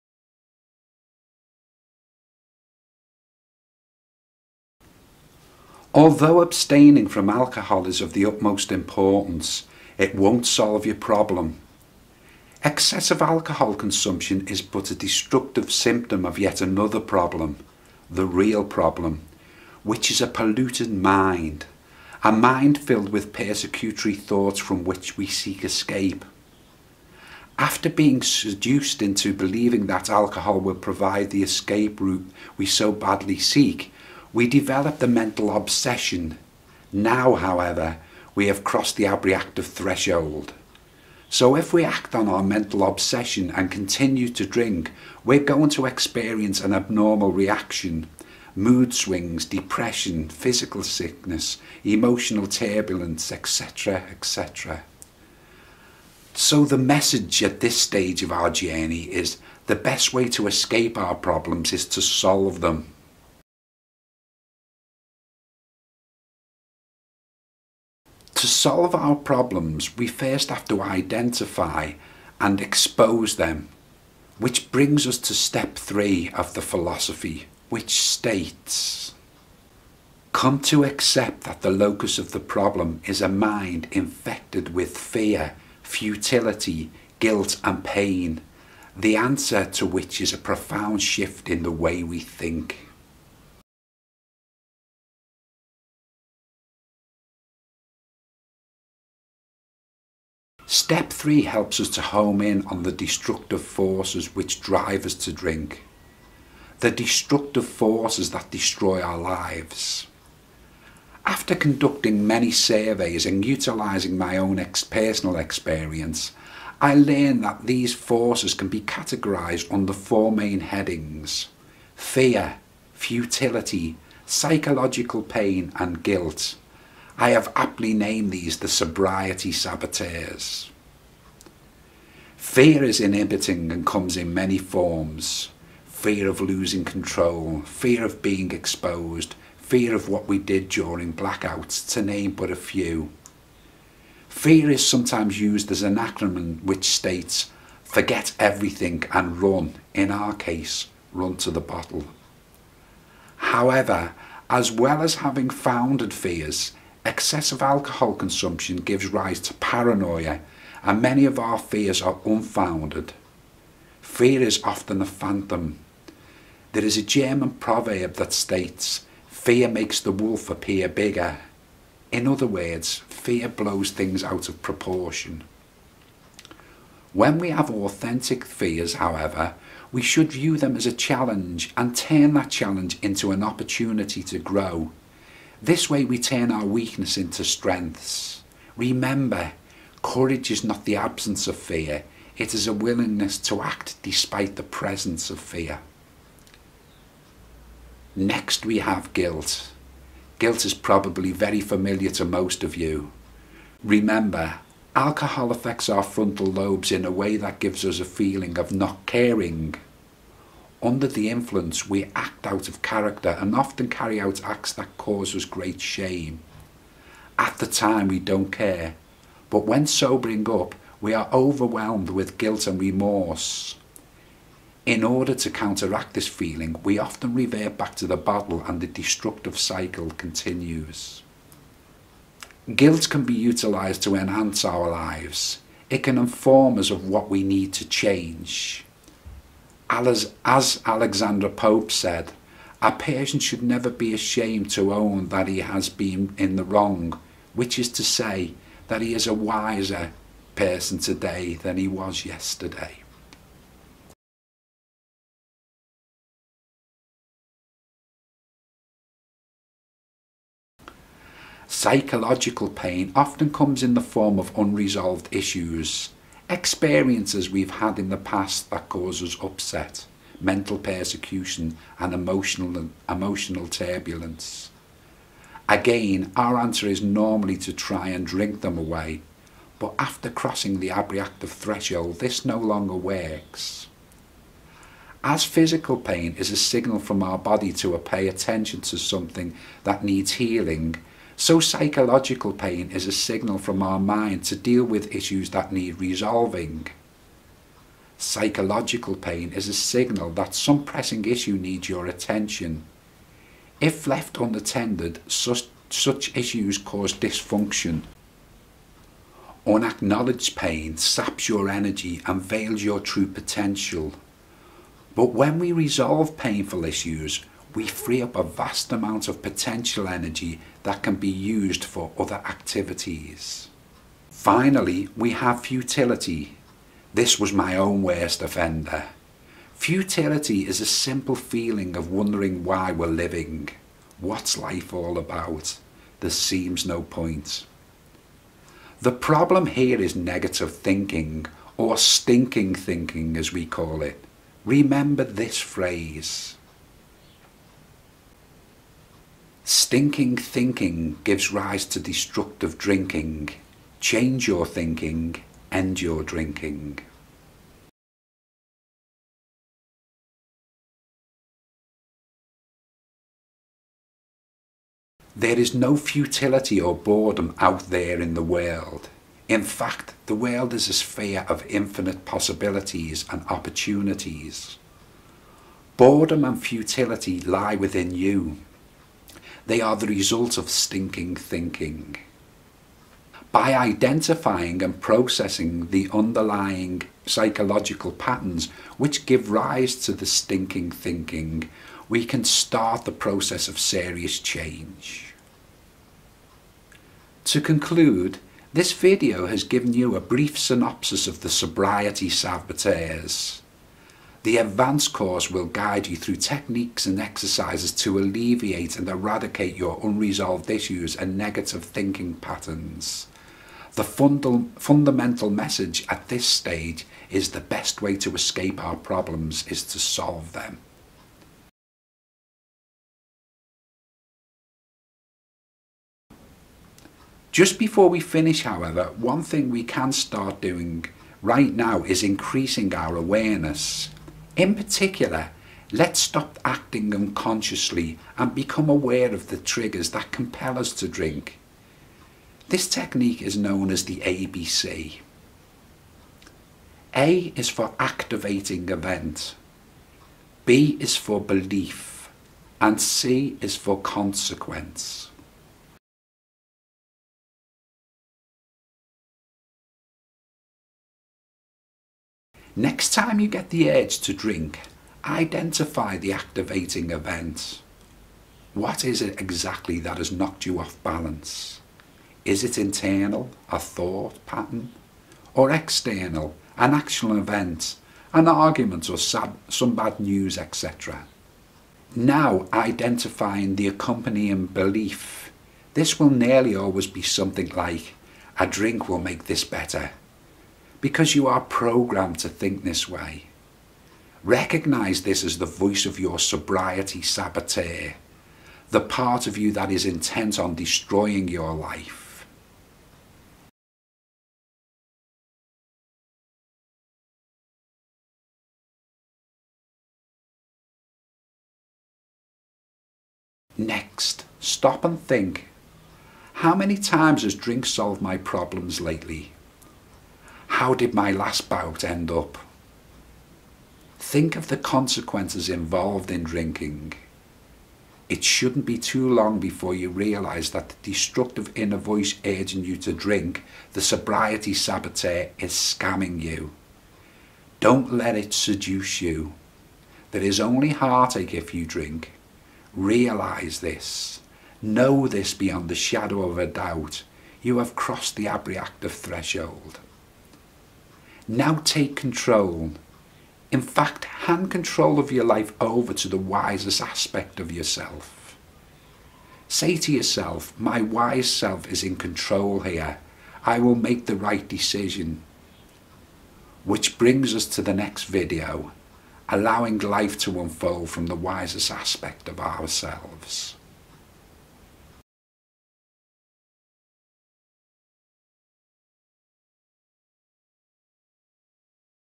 Escape Your Problems by Solving Them (5 of 9): Alcoholic to Alchemist - The Winners Way
Alcohol doesn't alleviate our problems, it simply masks them.

The best way to escape our problems is to solve them